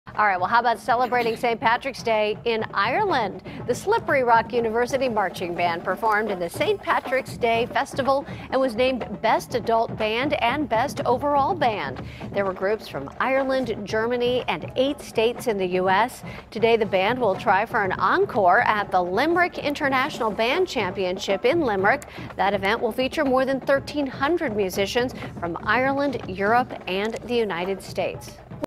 Slippery Rock Marching Band Named Best Overall Band In Festival In Ireland On St. Patrick’s Day
The Slippery Rock University Marching Band performed in the St. Patrick's Day festival and was named the best adult band and best overall band.